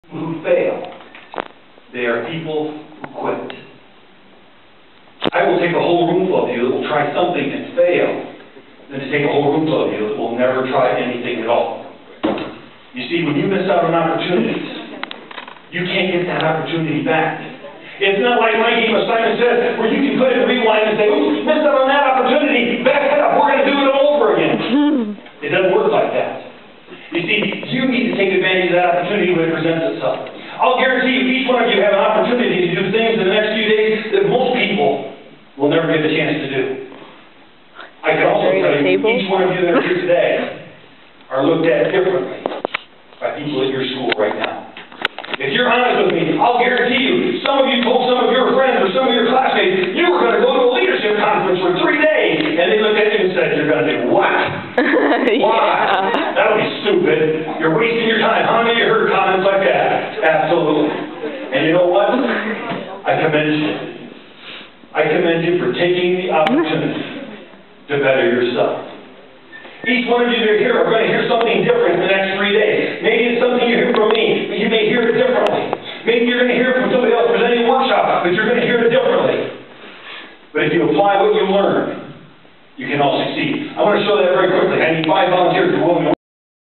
Rich talking about Opportunities
This was at the Skills Leadership Conference In Gettysburgh on November 17th. The day we wore pink for breast cancer. Rich is a 5 time world weight lifting champ and he came talk to us about Oppertunities...for this portion. Apoligies if cannot understand.